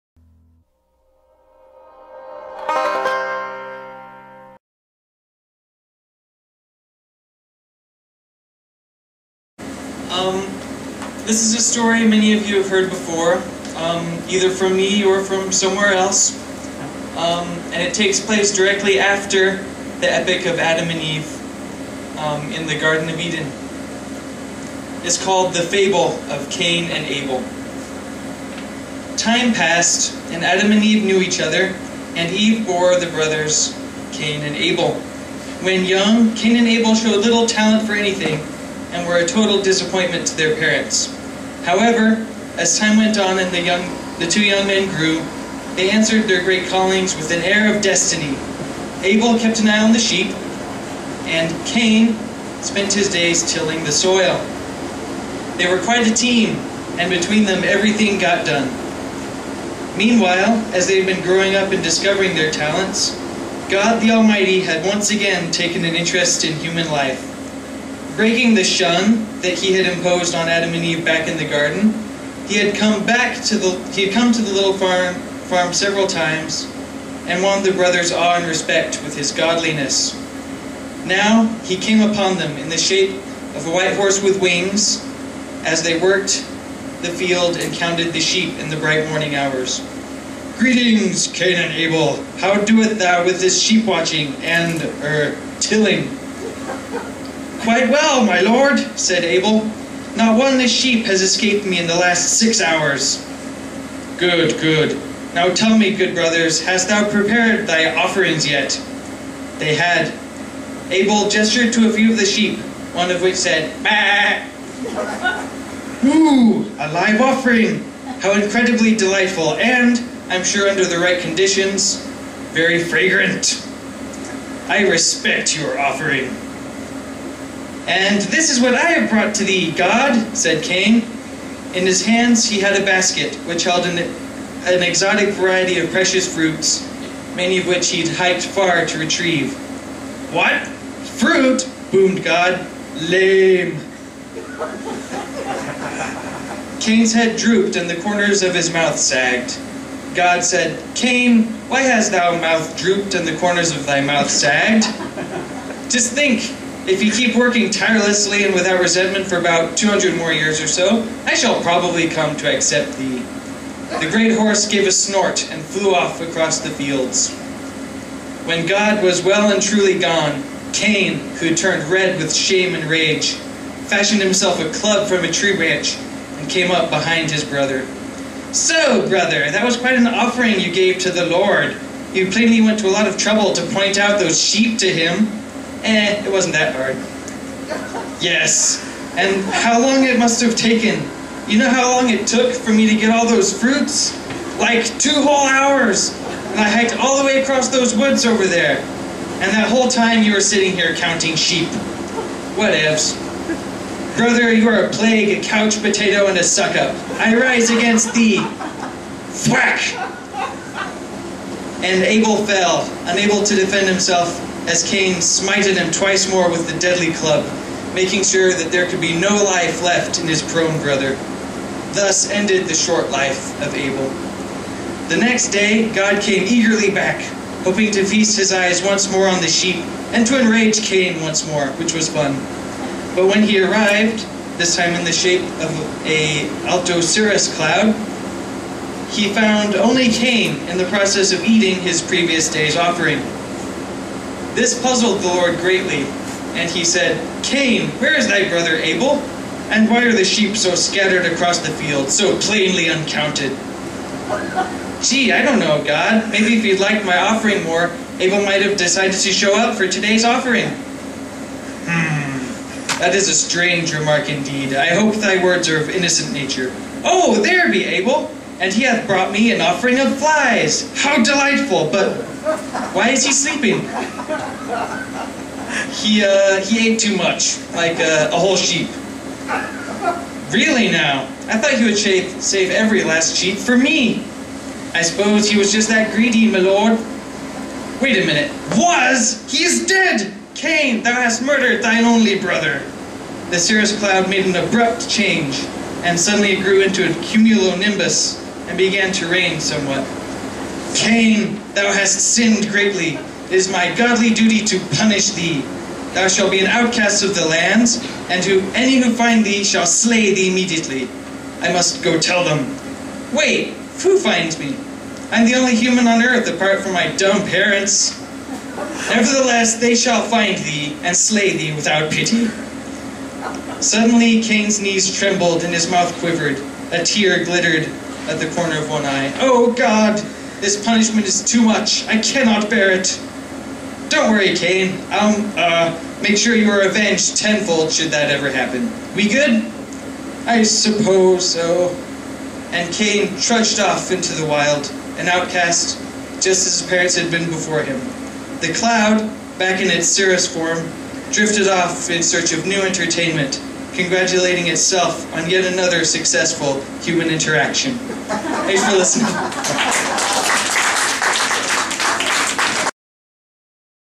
Pullmanfan7: The Fable of Cain and Abel
Pullmanfan7 reads his comical rewrite of the Cain and Abel story.